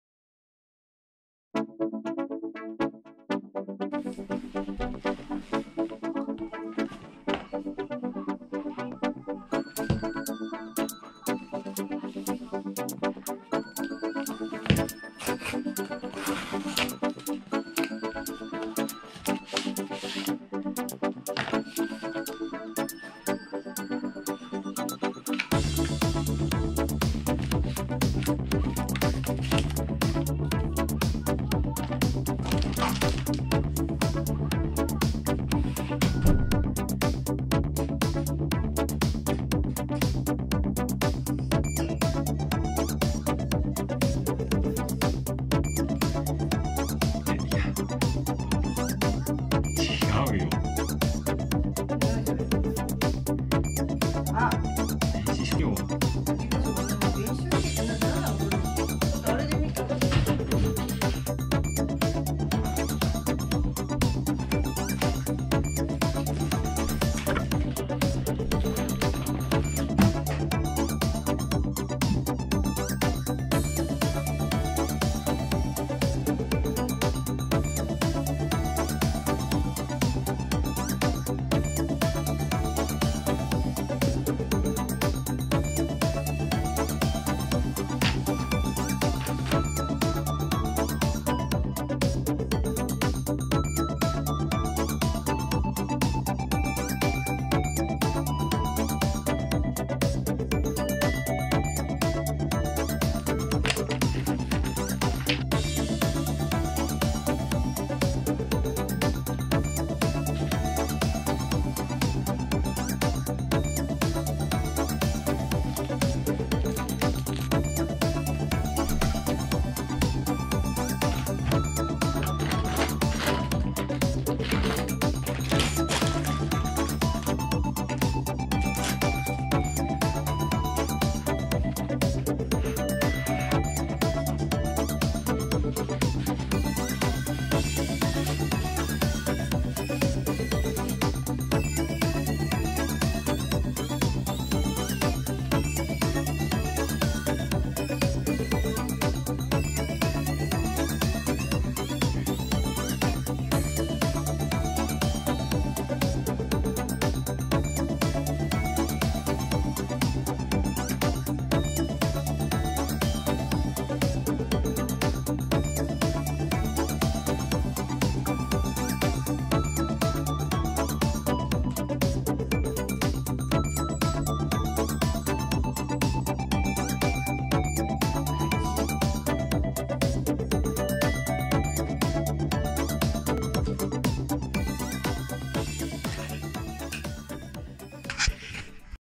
キンドルオアシス開封『電子書籍リーダー』
紙媒体から電子データに移行している現代社会において、漫画や小説などの娯楽物も当然のように紙からデータへと移っている。
今まで紙媒体で購入していたが、置き場所に困るようになり、自炊をするようになった。自炊したデータは電子書籍リーダーで読む必要があるので、
今回、『キンドルオアシス』を購入し、使用感を検討してみた。